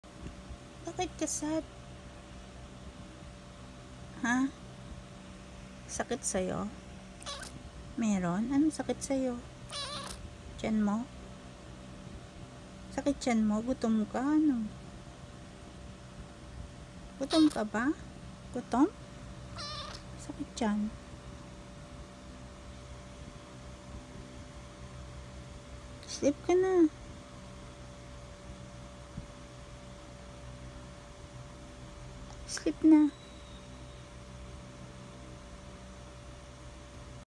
June 9, 2024
My cat (Apple)
is not feeling well